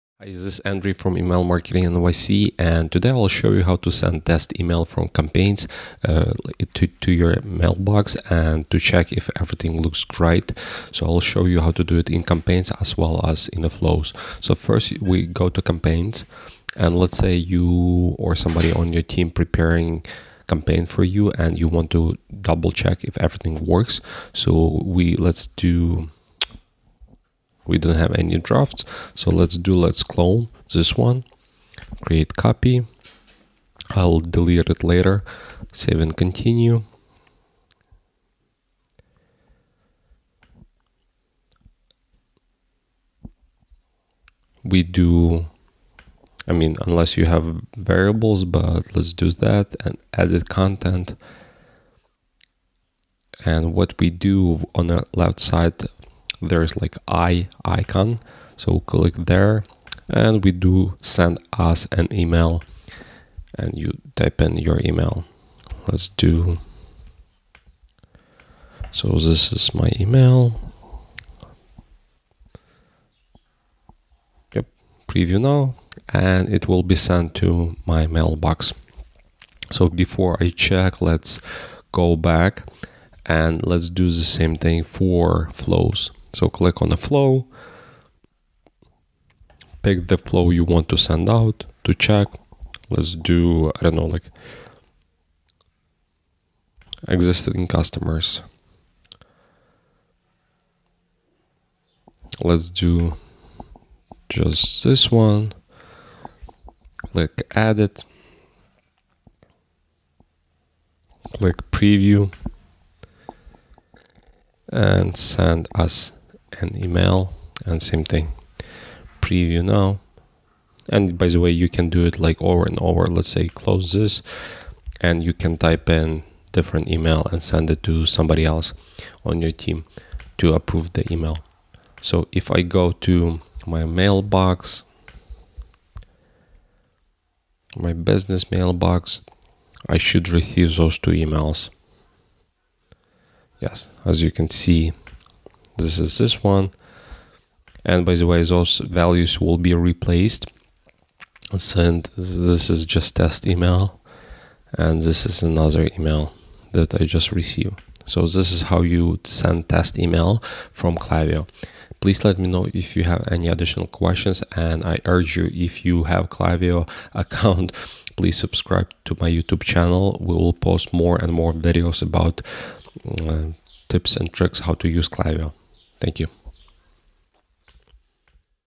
How to Send Test Email in Klaviyo
In this video, I will show you how to send a test campaign and test email from flow in Klaviyo